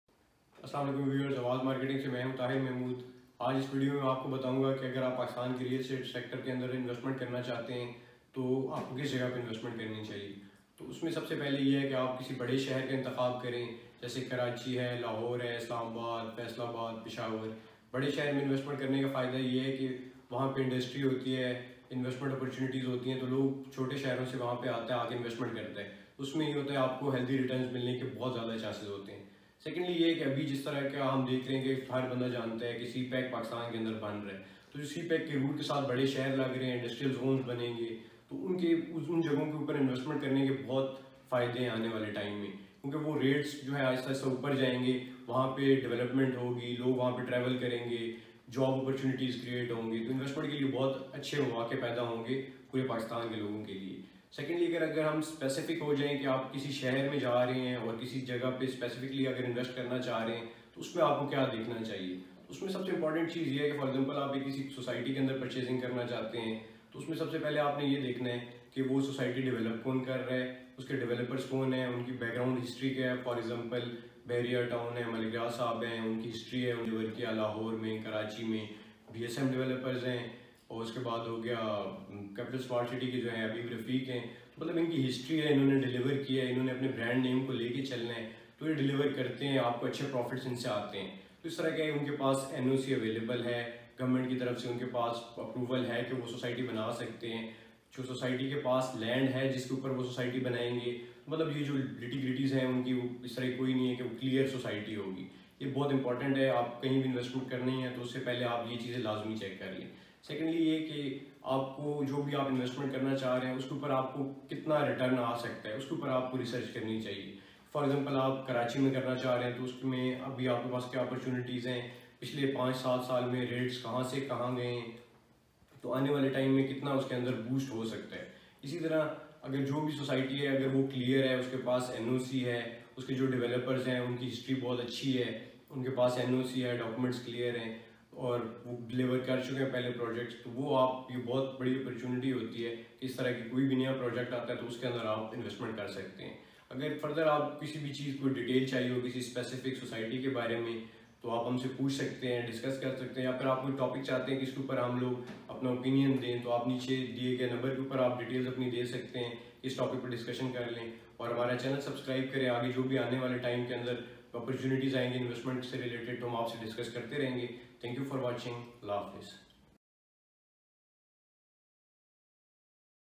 Where To Invest In Pakistan Real Estate Market.
A brief analysis given by Awaz Marketing expert on where to invest in Pakistan real estate market.
Our priority is to guide you towards the most profitable investment and become your "AWAZ".
             "  HAMARI PEHCHAAN APKI AWAZ"